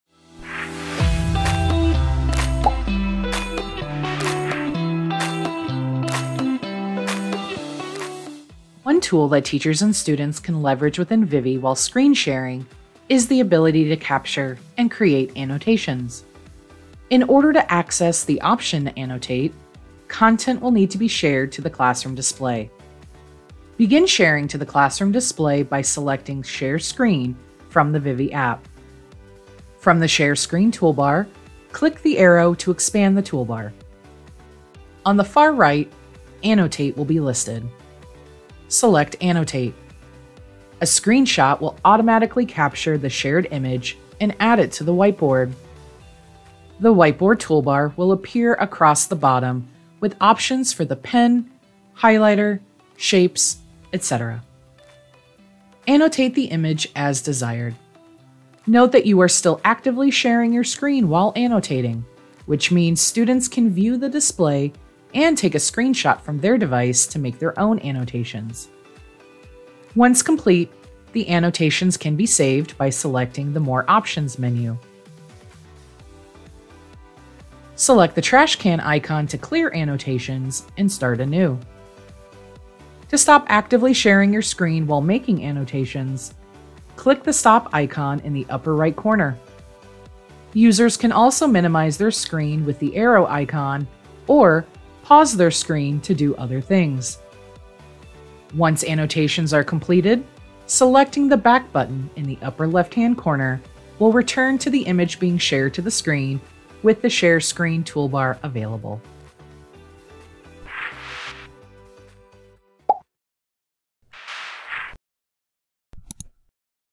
Annotate
Create engaging lessons while screen sharing through the Annotate feature with Vivi! Learn all the steps to optimize the effectiveness of screenshots and annotations in this quick video tutorial!